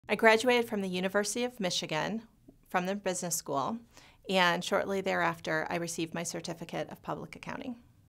Tax Advisor: Education - Karin Schmitz Career Girls Role Model
Tax Advisor Karin Schmitz shares valuable career guidance and life advice with girls.